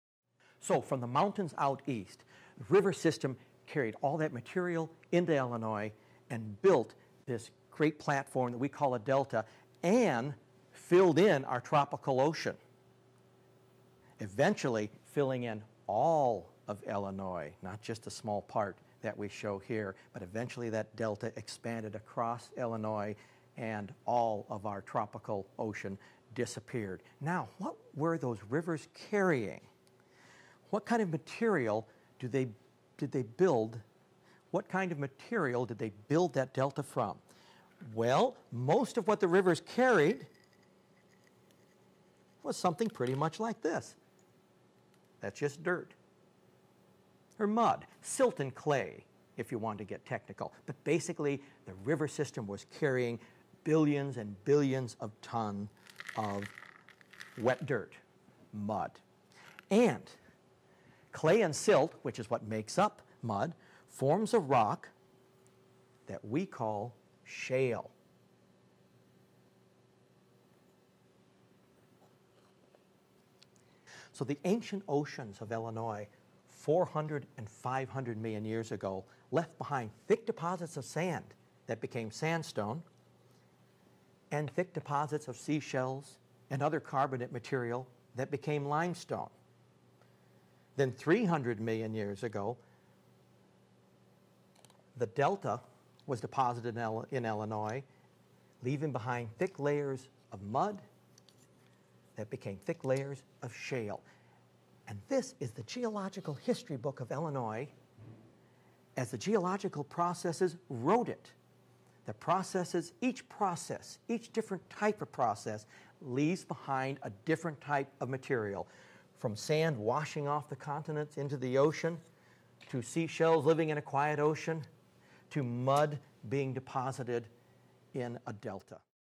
Chapter 4: Geologic Materials
Learn the history of Illinois as it changes from ancient tropical seas to towering swamps to a frozen Ice Age landscape!